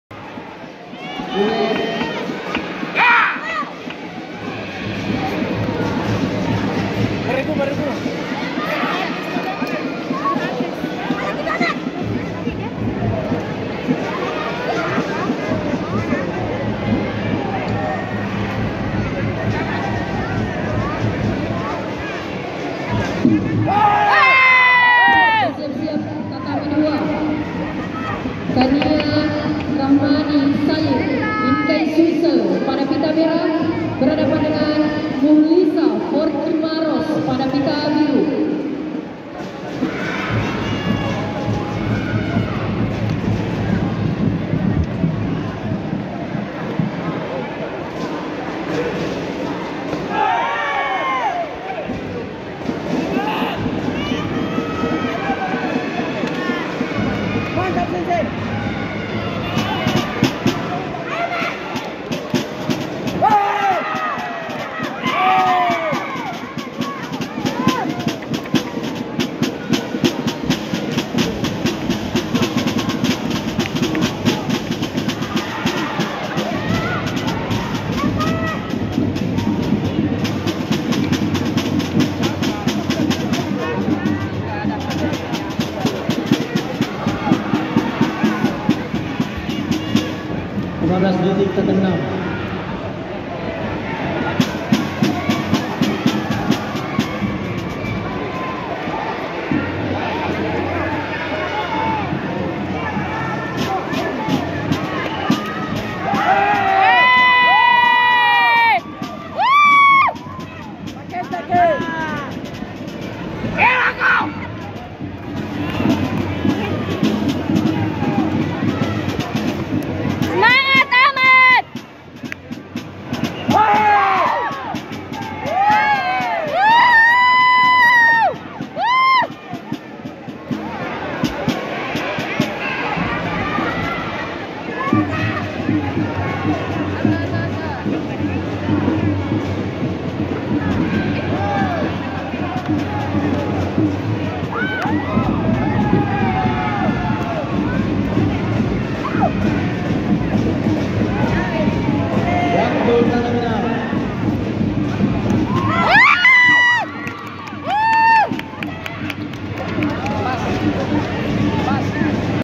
FINAL KUMITE -61 KG JUNIOR | Achmad Hamzah VS Muzalfa F | KEJURNAS KARATE OPEN SHINDOKA CUP I 2023
Final KUMITE -61 KG JUNIOR PUTRA pada kejuaraan Karate Open Tournament Tingkat Nasional & Festival SHINDOKA CUP I 2023 | Mempertemukan Achmad Hamzah (GOJUKAI SULSEL) VS Muzalfa Fatih (FORKI KALTIM) di JK Arenatorium UNHAS, Tamalanrea Makassar Tanggal 10-12 Maret 2023

Jika kalian suka videonya tekan tombol like dan share ke teman-teman kalian.
Terimakasih dan salam Fair Play

Quotes : "Seorang Juara Tidak Mengenal Kata Menyerah"